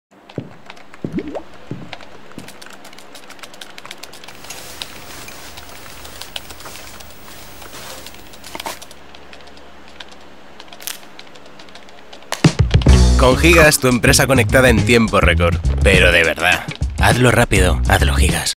GIGAS. CONECTADOS
Hazlo rápido, hazlo Gigas. Campaña de El Cuartel para la compañía internacional Gigas, especializado en la prestación de servicios convergentes e integrales de comunicaciones y cloud para empresas